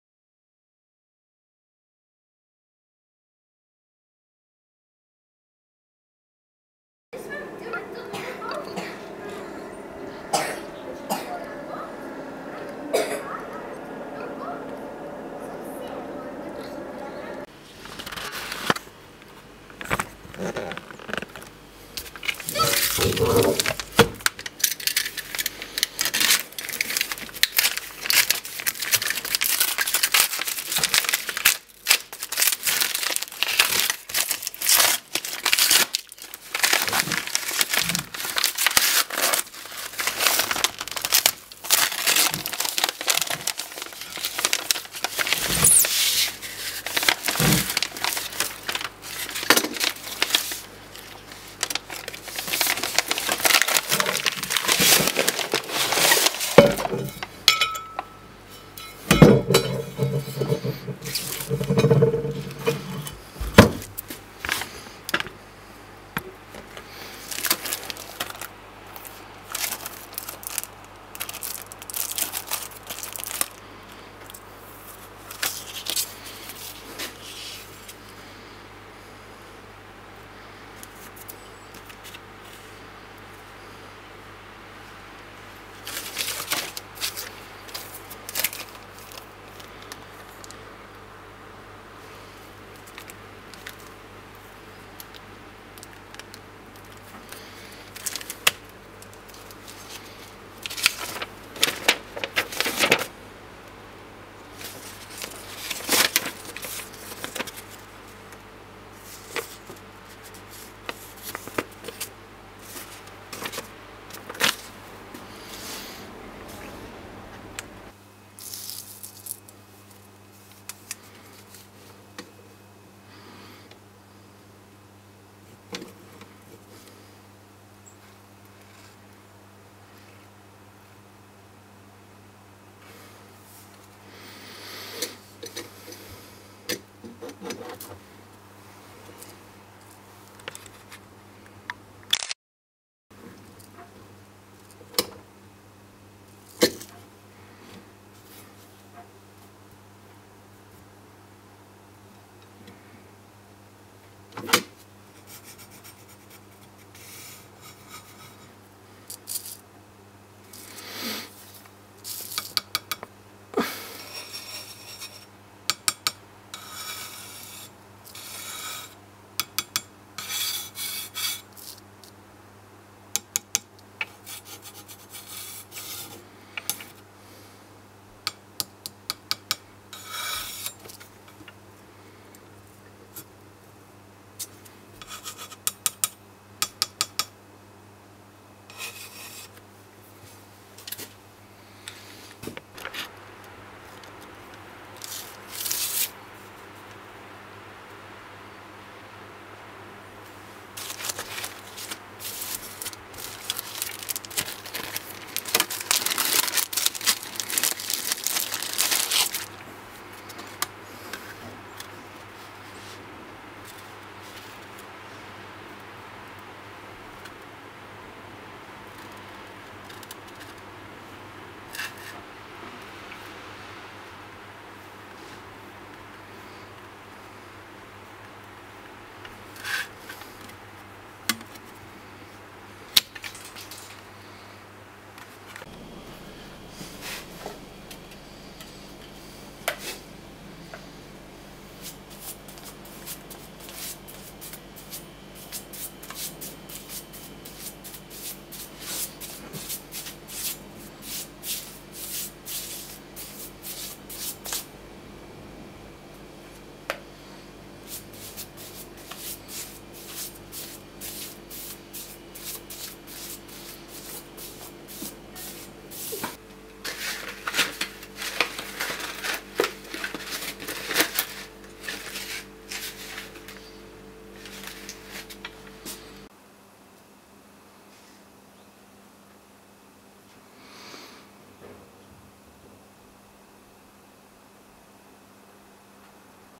Long Range Rondane nr 8 Gift in the mail / Gave i posten
Fikk uventet gave i posten i dag! Tusen takk til THLR og Vertebrae. Gleder meg til og prøve det ut! Føler jeg bare må ta testen på nytt og at det jeg presterte i film nr 5 er veiledende.
Nå som jeg har den riktige blinken må jo  "Ferdighetsmerke langhold", dvs 1 skudd mot 1 mål minst 500m unna, 1 minutt tilgjengelig, prøves på nytt.